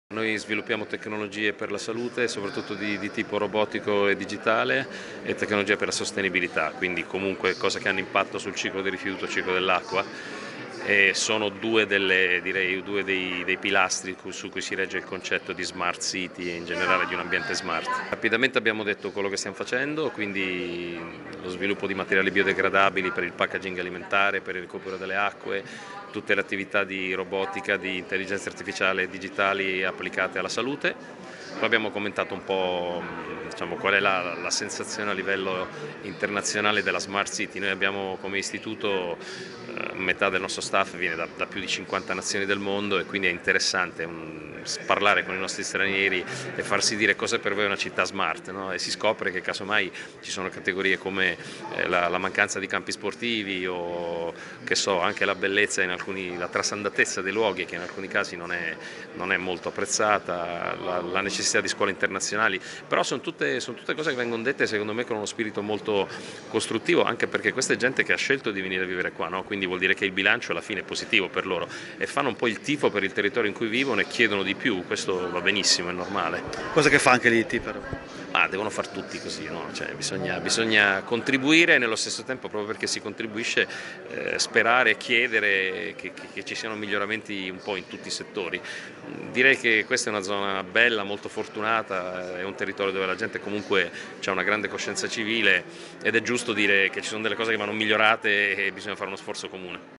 Roberto Cingolani, Direttore Scientifico, IIT
Sostenibilità, robotica e tecnologie medicali come pilastri per una Smart City; punti di forza e criticità del capoluogo ligure secondo i ricercatori stranieri dell'Istituto Italiano di Tecnologia.